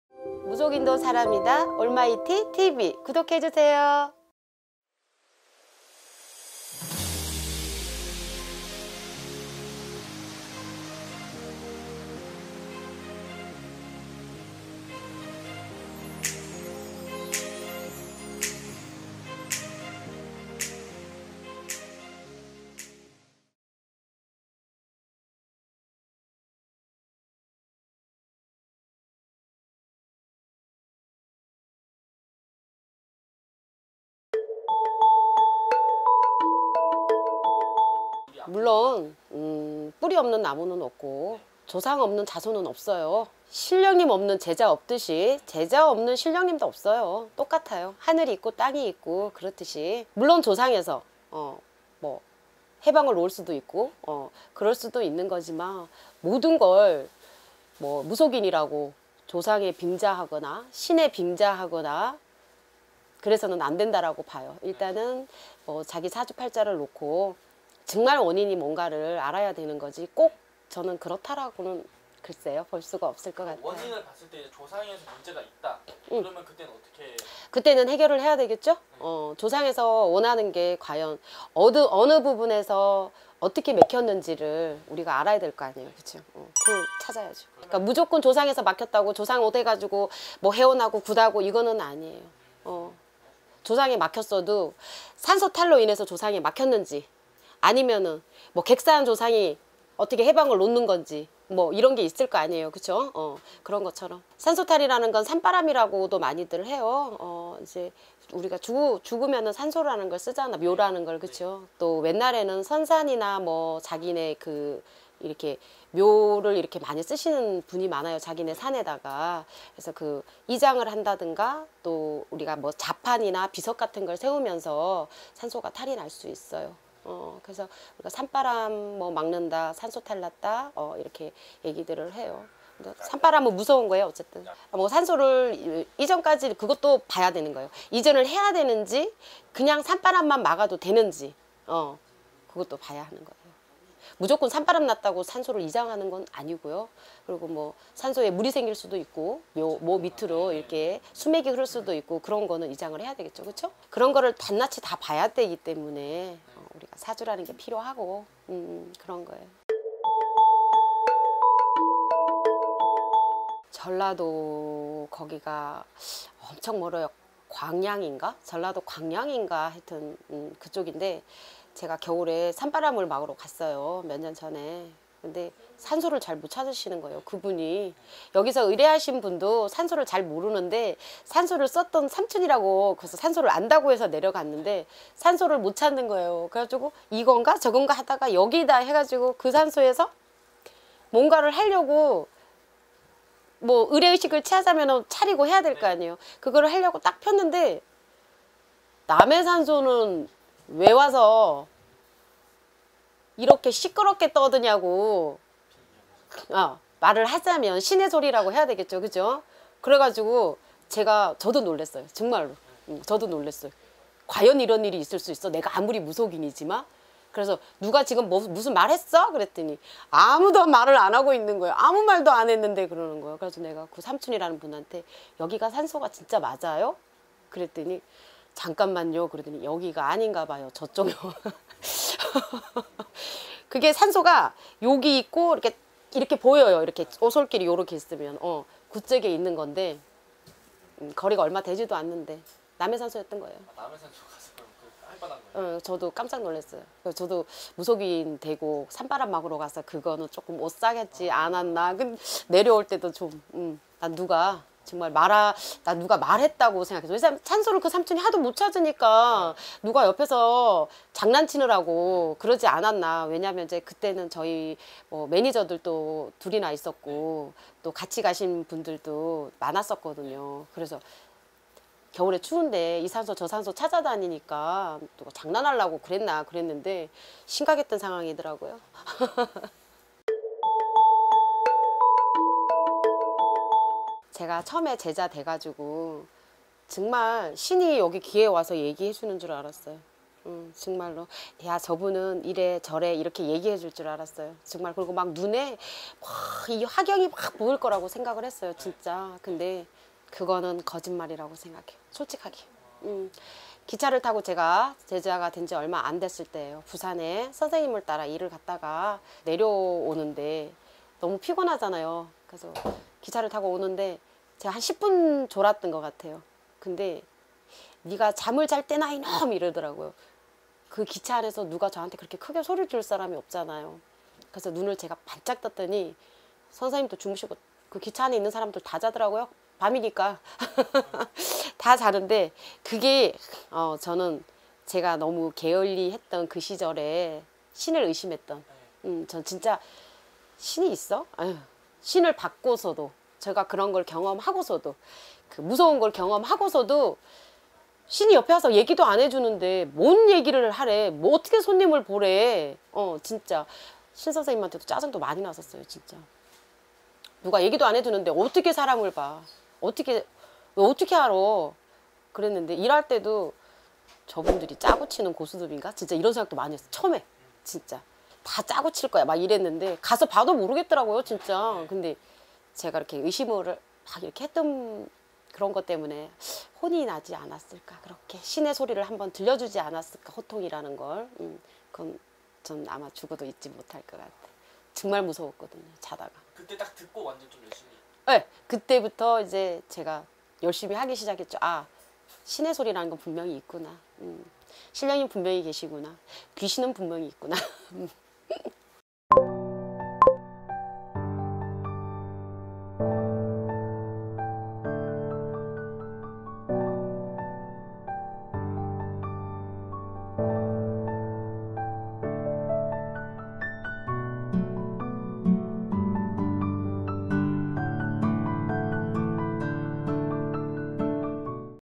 산소탈 중 무덤에서 들려온 의문의 소리?!.. 신을 믿지 않으셨던 선생님의 이야기 서울유명한점집 서울점집 강북구점집 하백신당
자막
★서울점집 하백신당★

★대표블로그★
-올마이티TV

★대표블로그★
-올마이티TV